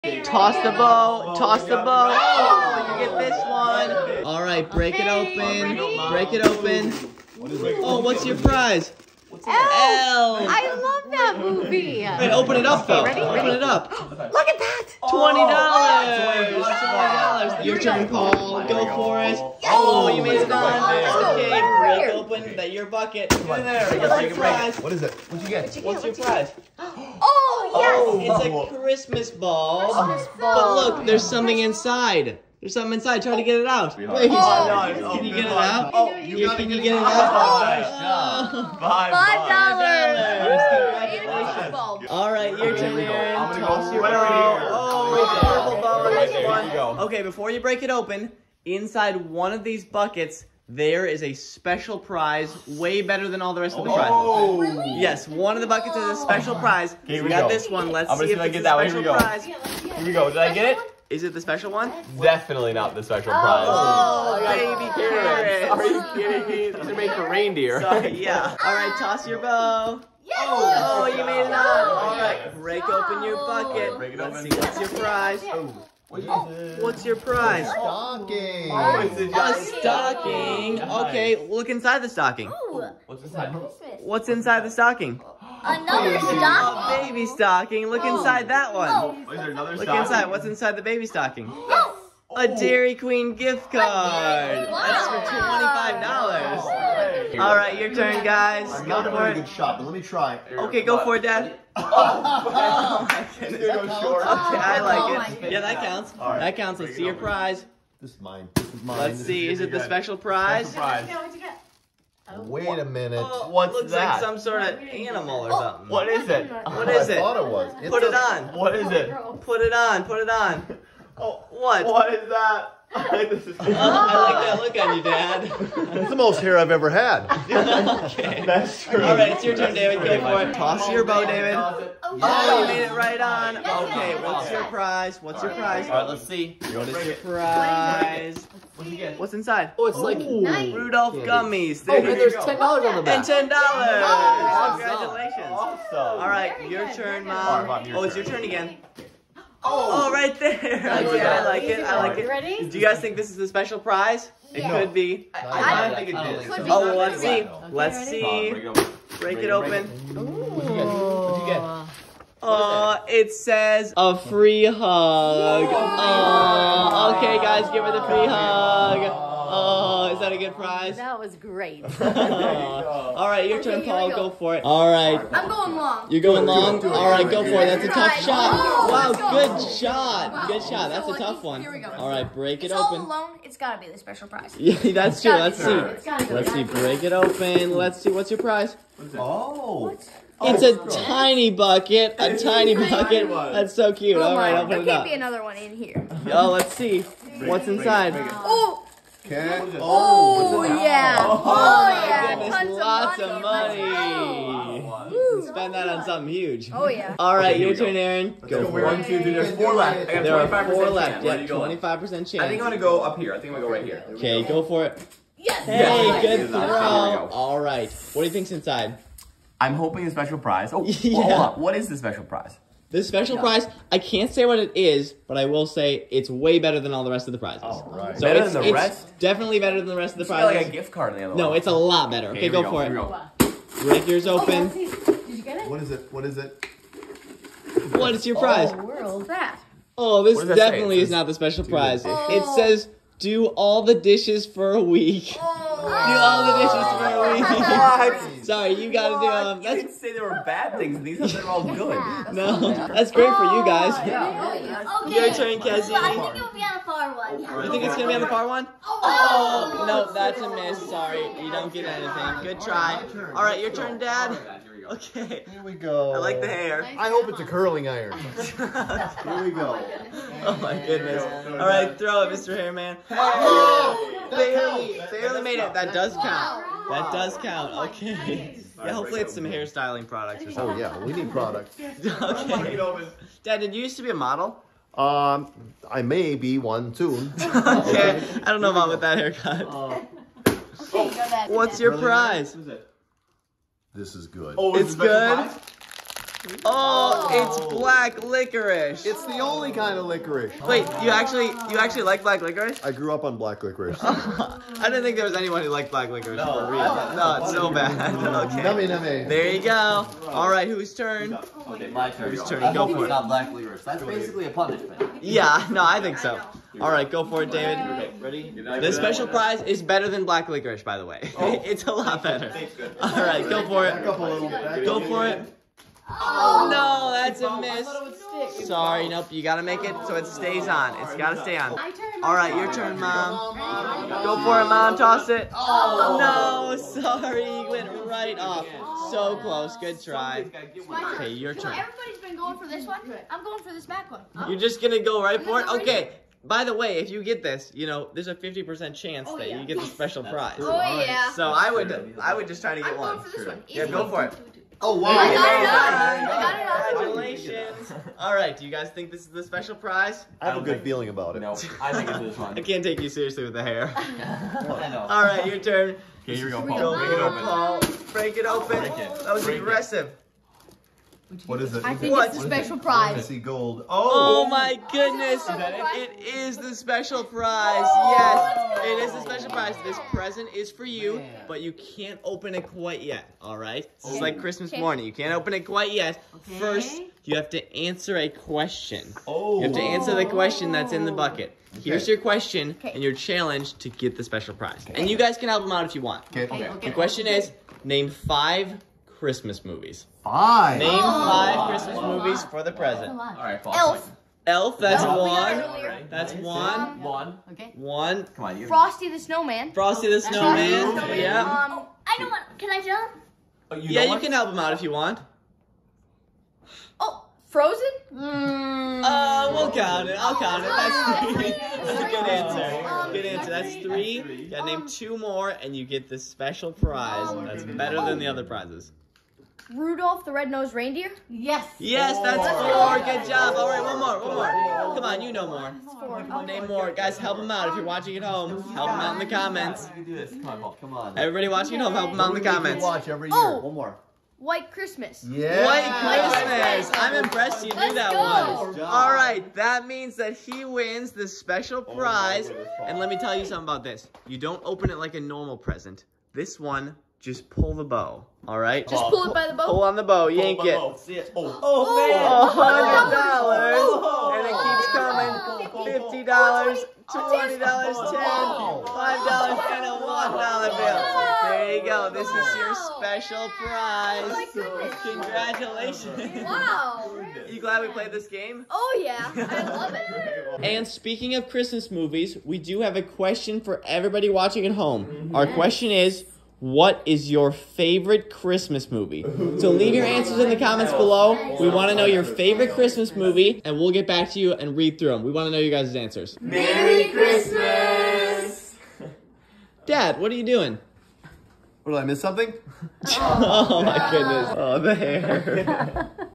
Toss the Bow, Win a Prize -- Christmas Game 🎁
Dad, mom, kids, and the whole family play Christmas bow tossing game for fun prizes. Toss your gift bow on the right bucket, win a special golden prize.